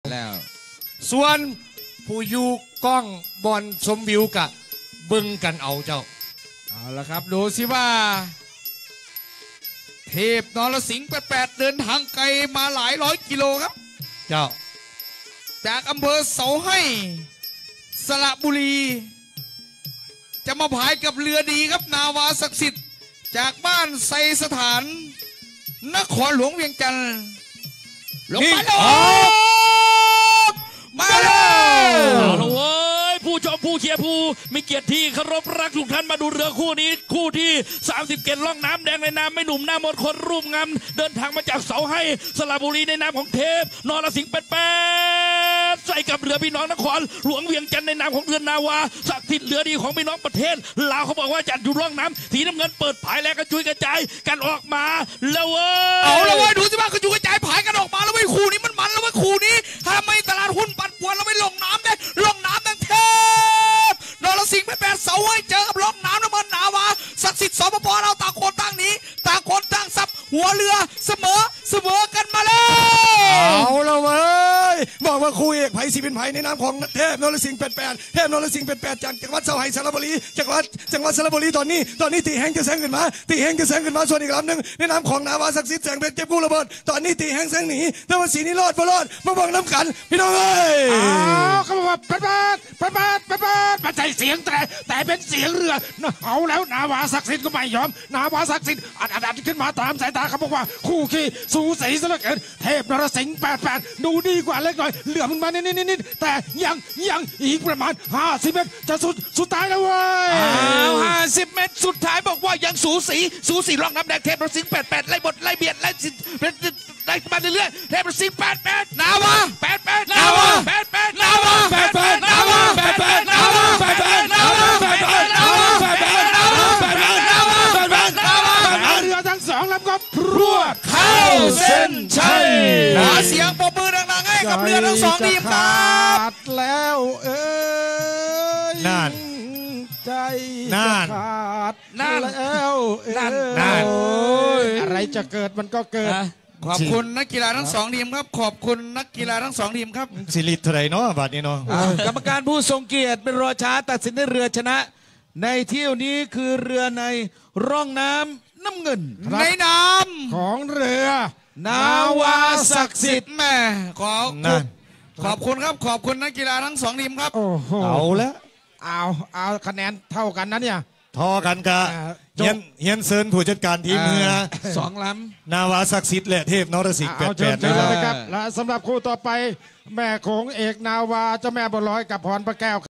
ดุเดือด เทพนรสิงห์88🇹🇭(ปุ้ย+นาล้อง) vs นาวาสักสิด🇱🇦(พันแสง+นาวาสักสิด) จ.หนองคาย 24.ต.ค. 2566
การแข่งขันเรือยาวประเพณีไทย-ลาว🇹🇭🇱🇦 ท่าเสด็จ จ.หนองคาย 24-25 ต.ค. 2566
55 ฝีพาย สาย B เที่ยวกลับสายน้ำ
Cr. ไทยลองโบท TLB